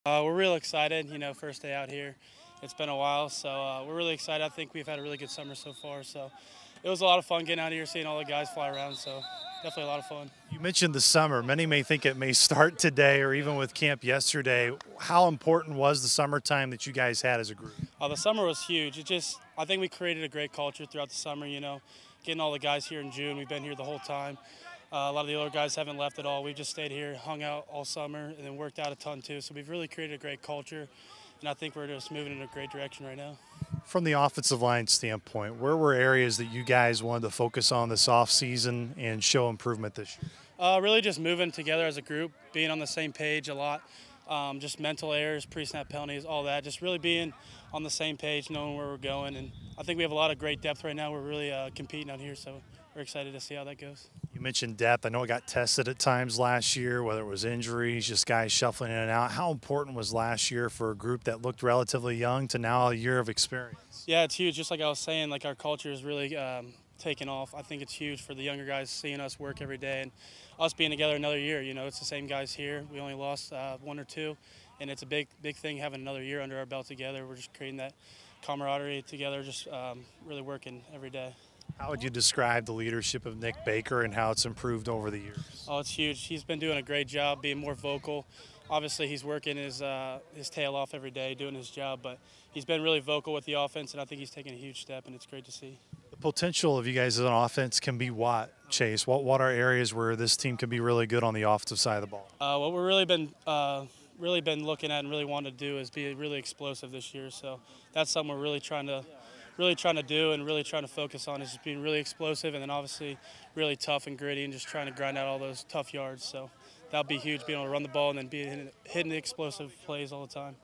8.3.23 | Post Practice Interview - Chase Evans
Offensive lineman Chase Evans recaps the first day of Saluki Fall Camp 2023.